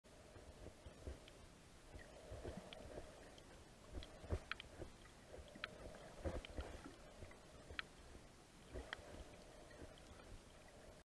Gray Snapper Swimming at Bahia Honda State Park
Gray snapper (Lutjanus griseus) swimming among the seagrass at Bahia Honda State Park. Florida Keys.